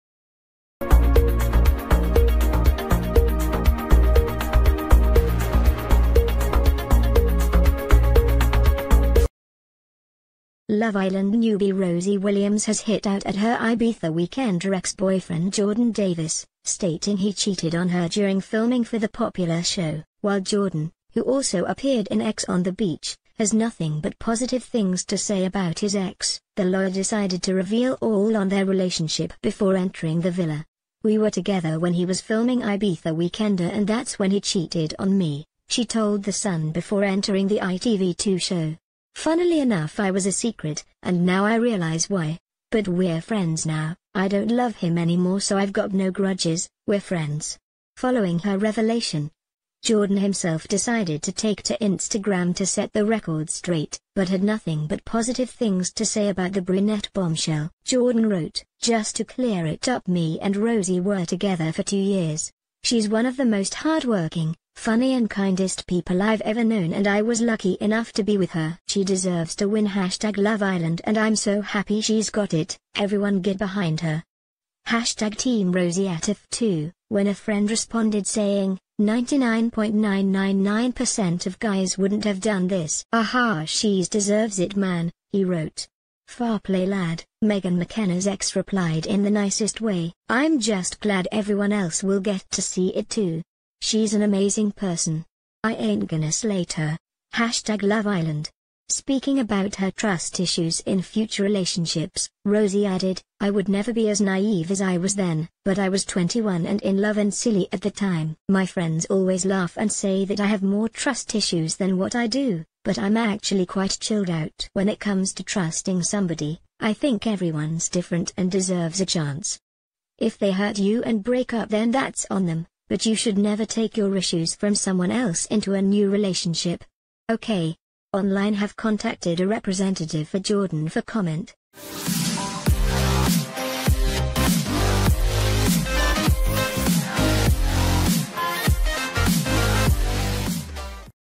Love Island: Rosie Williams brands Ibiza Weekender ex Jordan Davies a CHEAT – while he claims she's
Love Island: Rosie Williams brands Ibiza Weekender ex Jordan Davies a CHEAT – while he claims she's 'amazing' and should win the show
Love Island newbie Rosie Williams has hit out at her Ibiza Weekender ex-boyfriend Jordan Davies, stating he “cheated” on her during filming for the popular show.
While Jordan – who also appeared in E